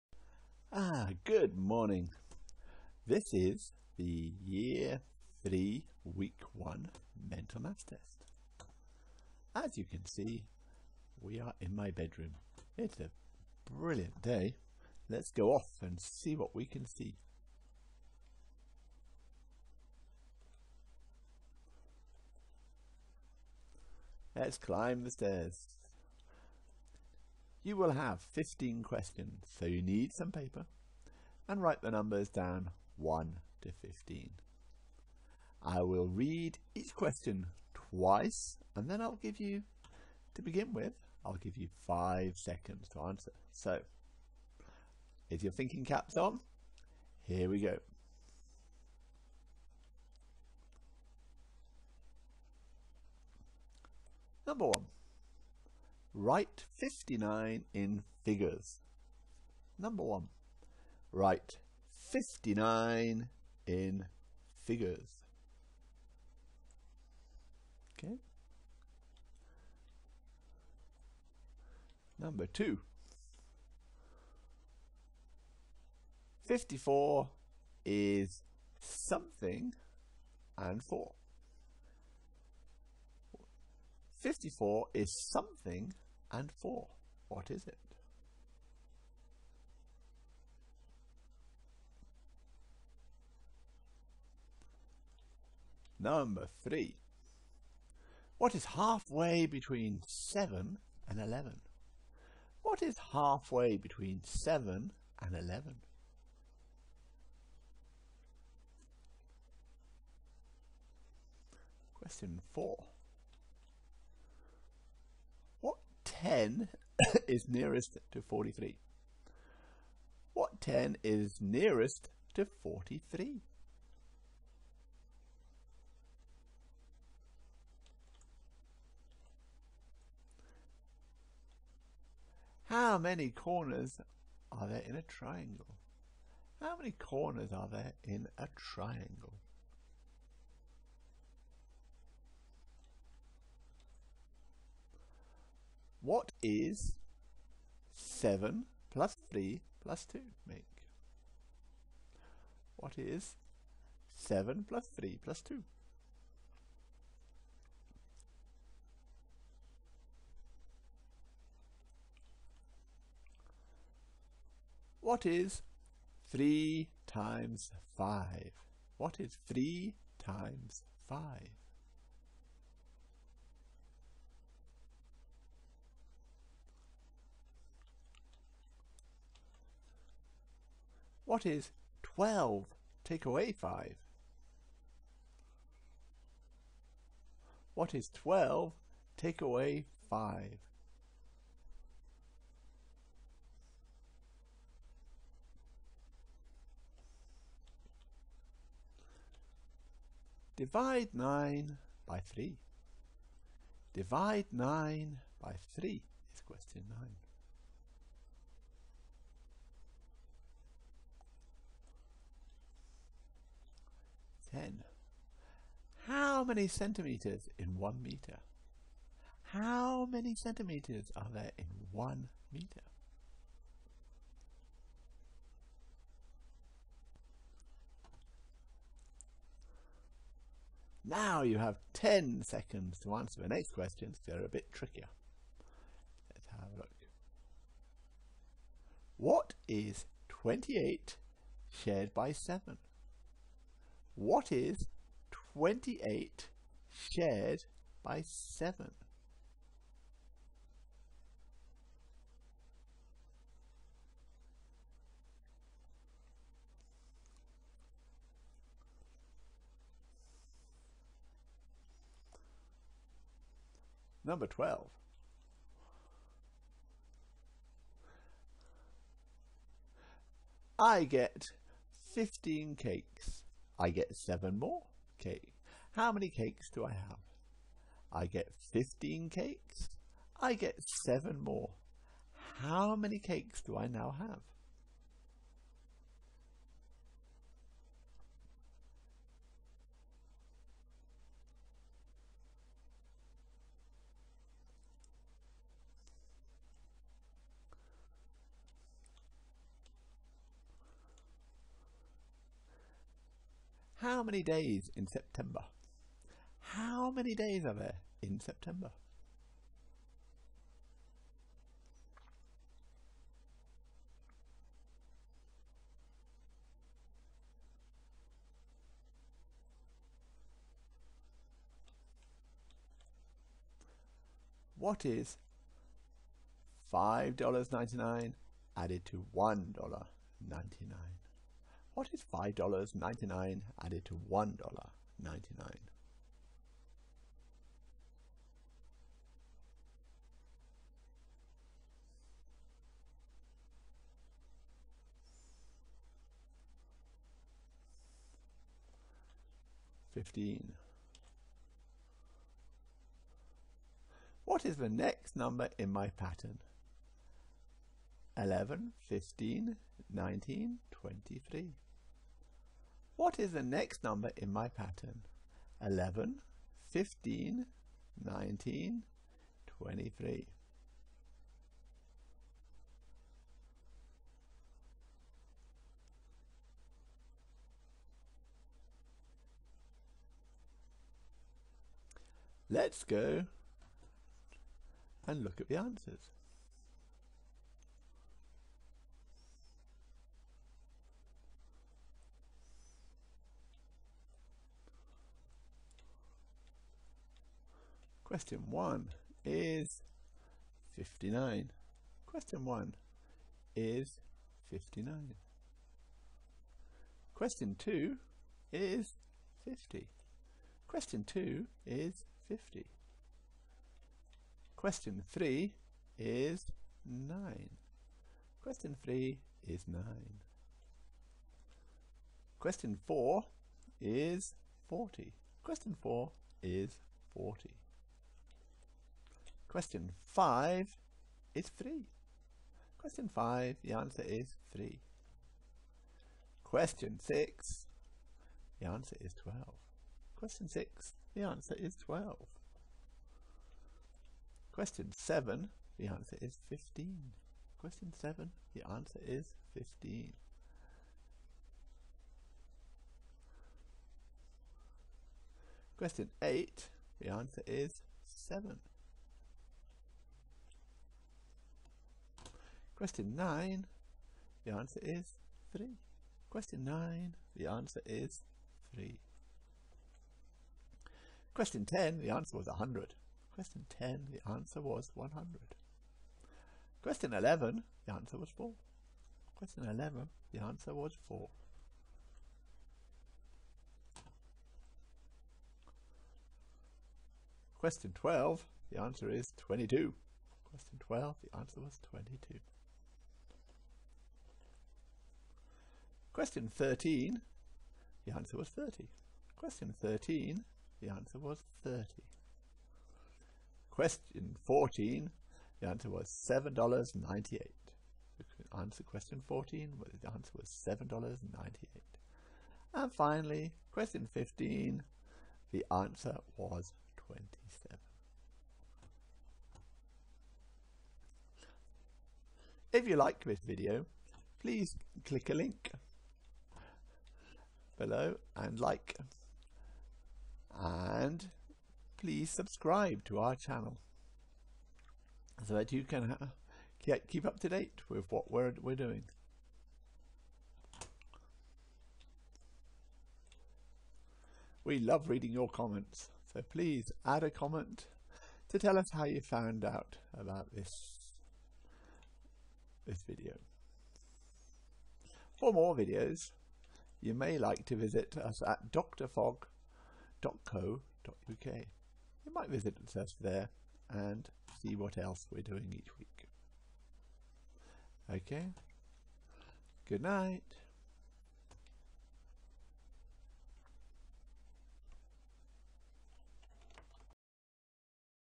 Year 3 weekly mental maths assessment
Year 3 weekly mental maths assessment is a great way of getting children excited about maths.